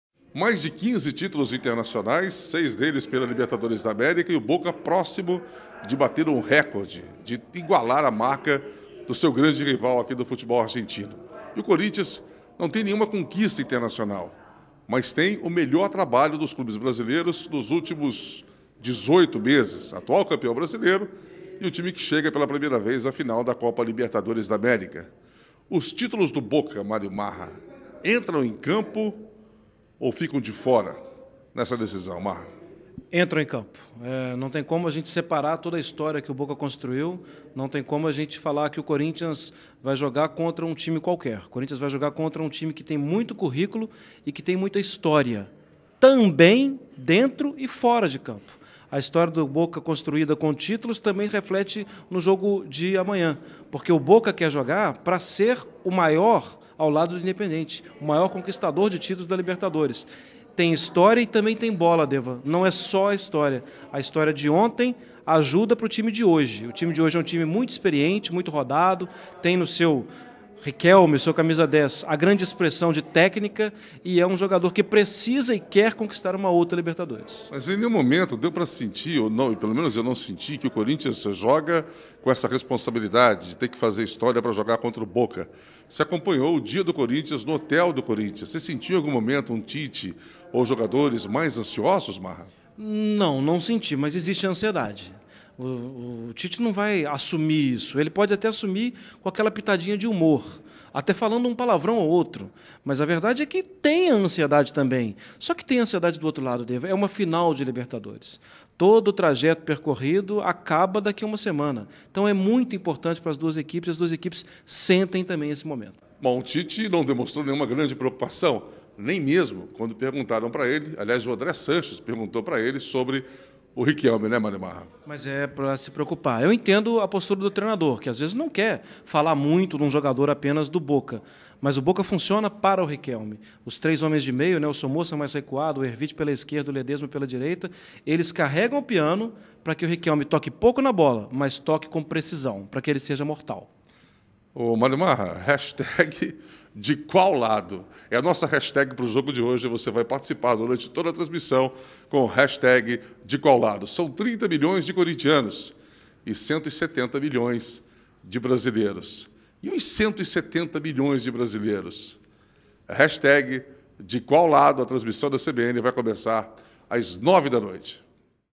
Deva e Marra analisam final da Libertadores entre Boca e Corinthians
Narrador e comentarista transmitem Boca x Corinthians, direto de Buenos Aires, às 21h desta quarta-feira, para todo Brasil.

Participe da transmissão pelo twitter, seguindo o perfil @futebolcbnsp e utilizando a hashtag

Aliás, DEqualLADO você está? Ouça a chamada do jogo